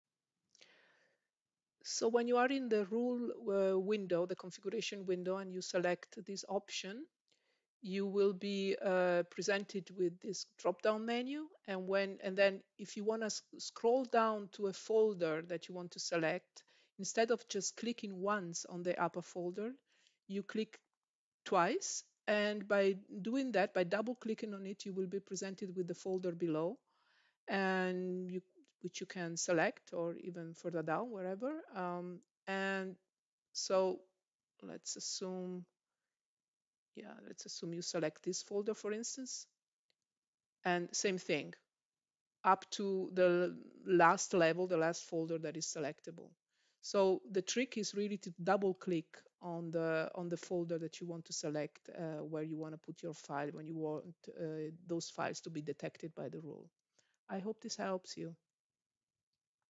SDL WorldServer - How to navigate through a folder directory to configure your Business Rule
to get product help, find best practices and share your knowledge with peers.